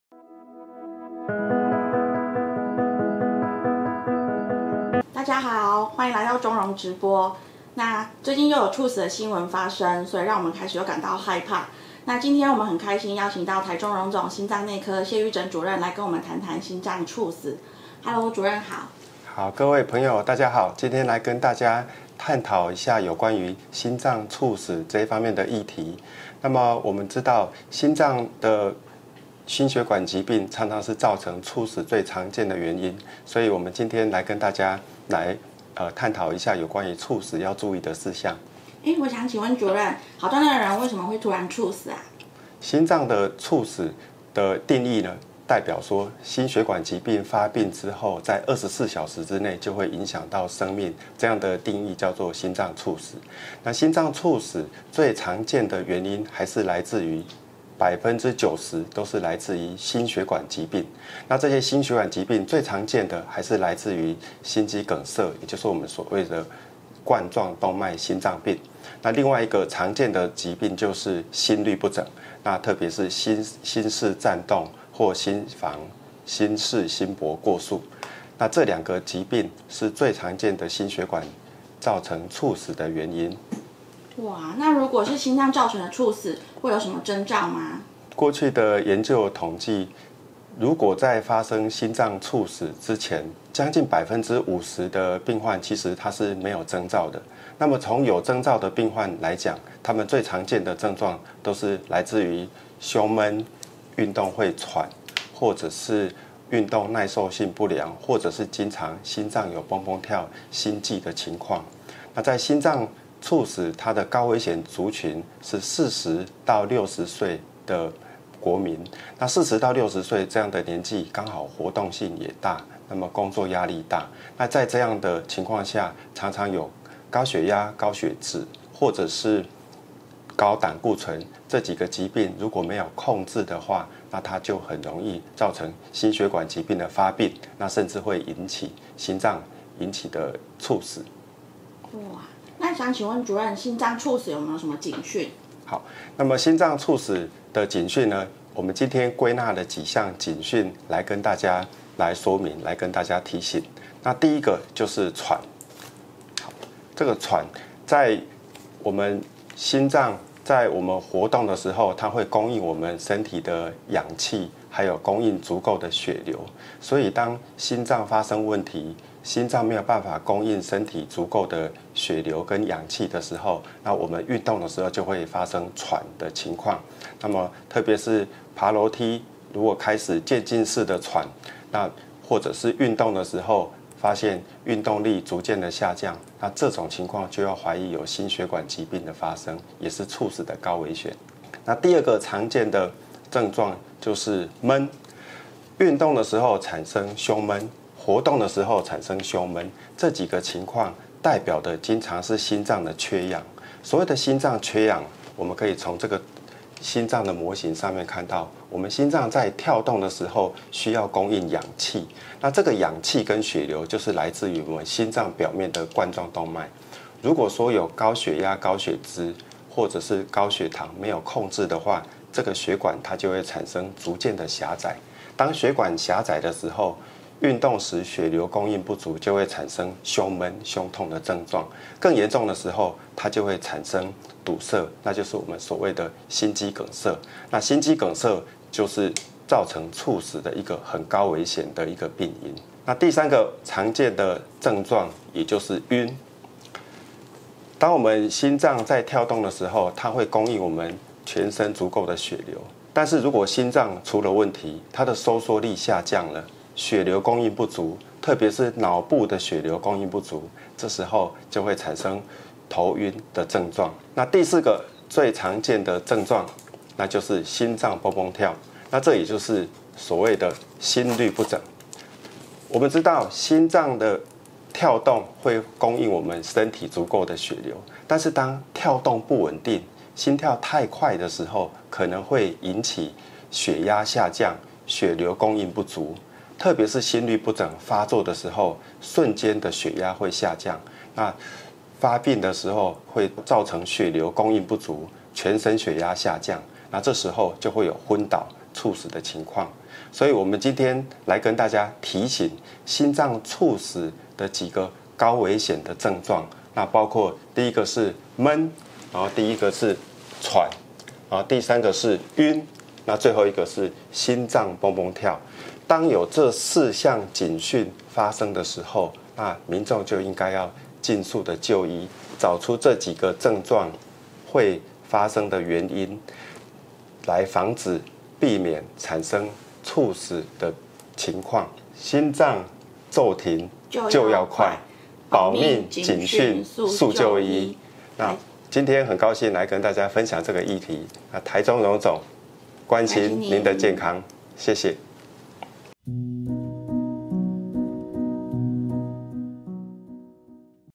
✪心臟驟停救要快 保命警訊速就醫✪
有哪些狀況會造成猝死呢? 有什麼徵兆或警訊呢?
心臟內科謝育整科主任線上為您解惑~~

Q1 好端端的人為什麼會猝死啊?
Q2 那如果是心臟造成的猝死，會有徵兆嗎?
Q3 請問心臟猝死的警訊是什麼?